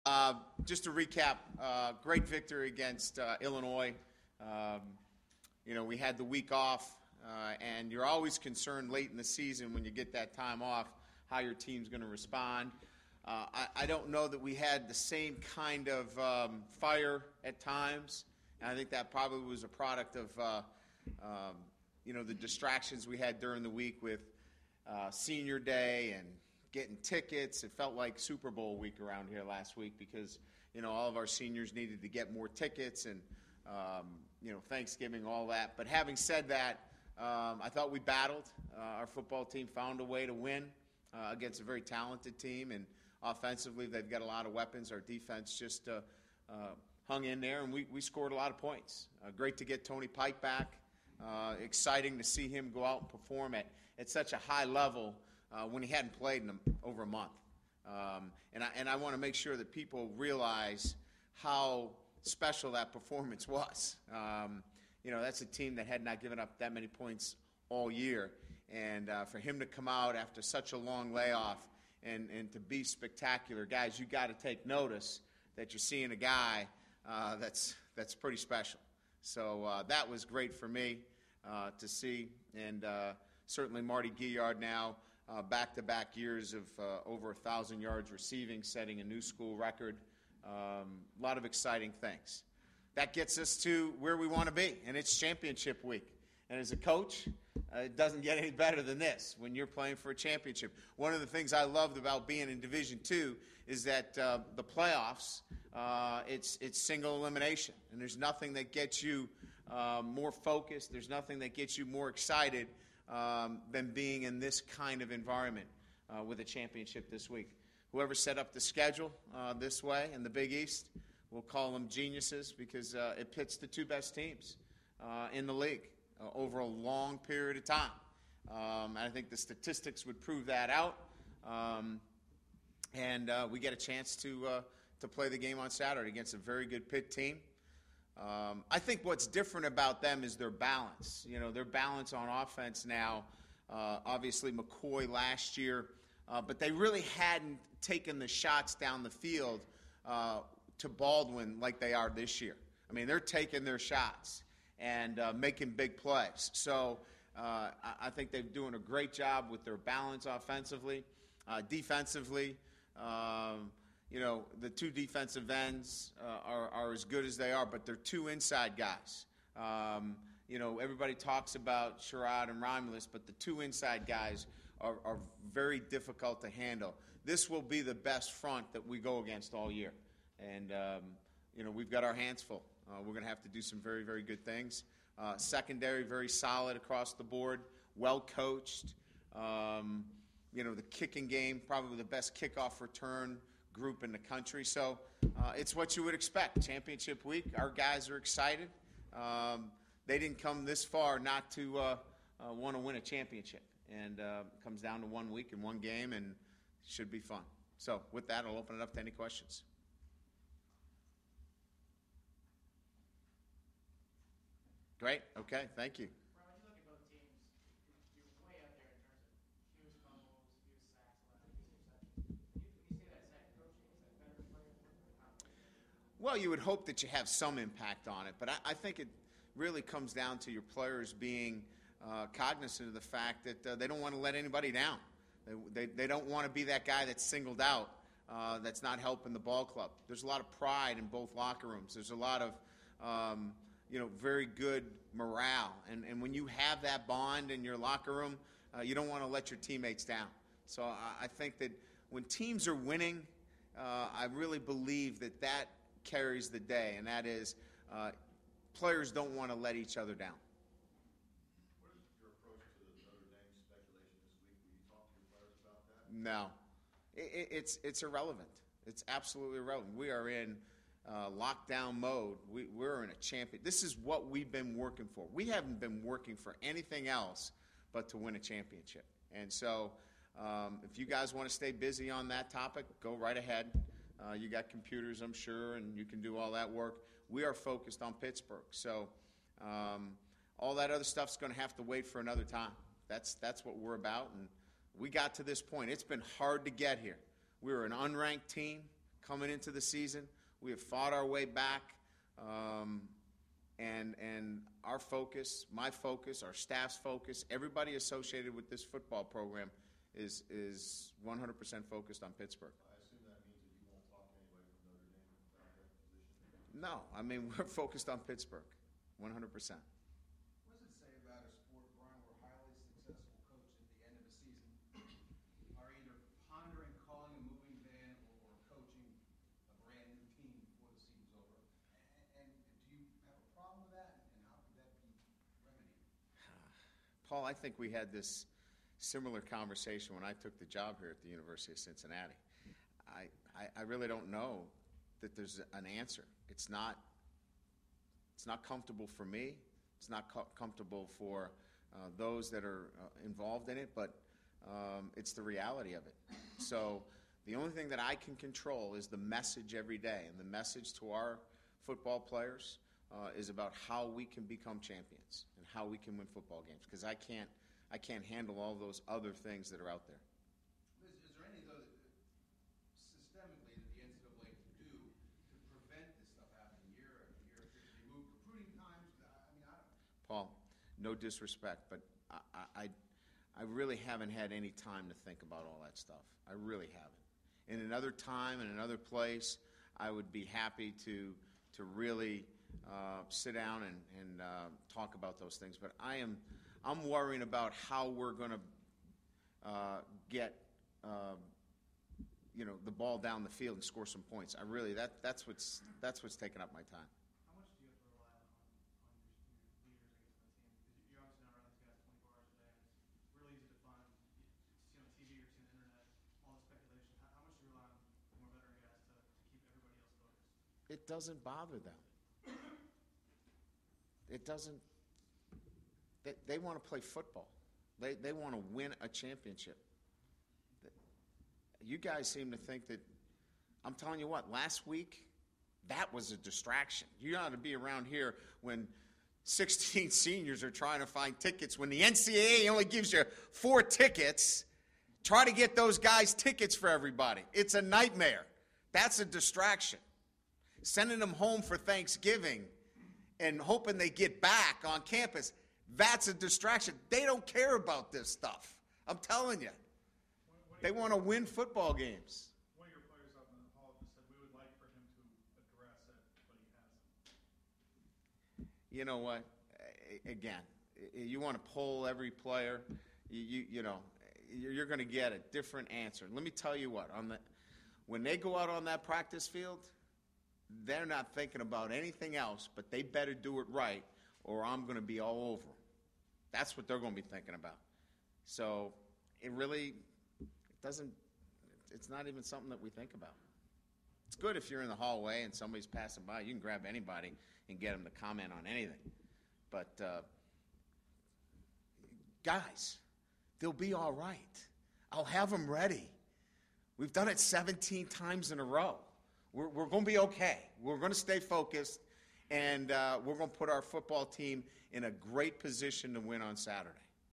Kelly Addresses Notre Dame Questions At News Conference
Bearcats coach Brian Kelly said a lot to say nothing about the Notre Dame opening Tuesday.